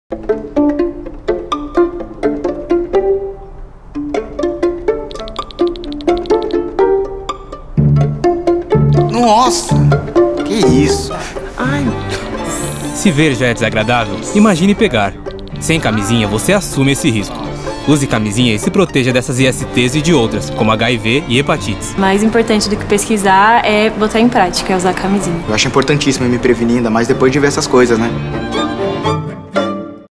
nova/sb - Portfólio - Ministério da saúde
A campanha nacional de prevenção às Infecções Sexualmente Transmissíveis (IST) do Ministério da Saúde tem como conceito “Sem camisinha, você assume esse risco”. Usando o formato “react”, bastante conhecido nos vídeos vistos pelos jovens, a campanha busca instigá-los a procurarem na Internet por imagens dos sintomas de algumas dessas infecções para terem uma real noção do que pode acontecer com quem não se previne. Nas peças, aparecem as reações ao verem imagens de doenças que podem ser prevenidas com o uso da camisinha, além da Aids, sífilis, gonorreia, cancro mole e herpes genital.